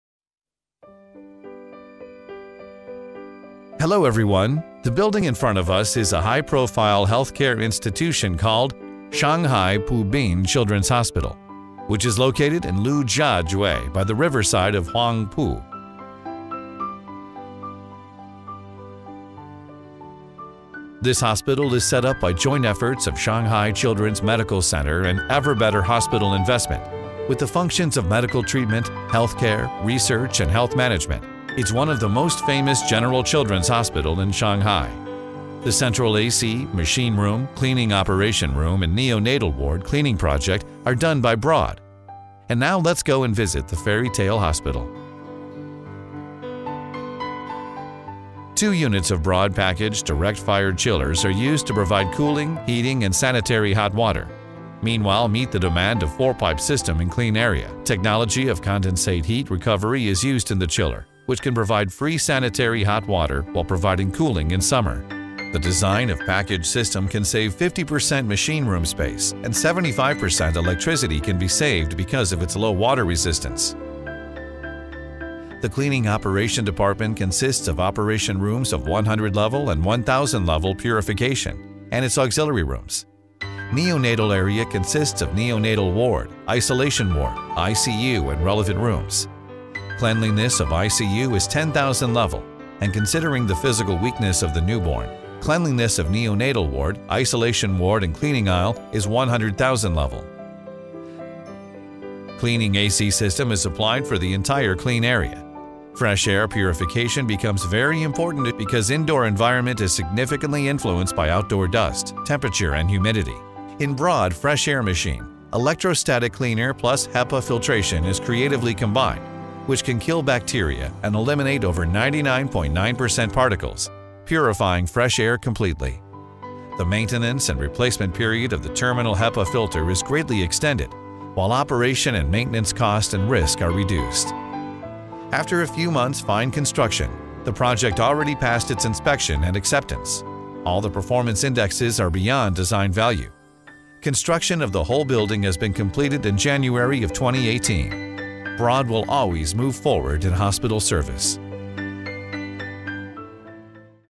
BROAD Group Clean Room Engineering Project | Shanghai Pubin Children’s Hospital
BROAD products combine to provide energy-efficient and clean facilities at the Shanghai Pubin Children's Hospital

Indoor air quality is of vital importance at the hospital, where strict cleanliness levels in the neonatal, ICU, isolation ward and other facilities must be met. BROAD's Clean Fresh Air machines eliminate harmful bacteria and PM2.5 particles, providing indoor air that is completely purified.

In the machine room, two BROAD packaged, direct-fired chillers provide cooling, heating and sanitary hot water, all with far greater energy efficiency and on a much smaller footprint than traditional HVAC units.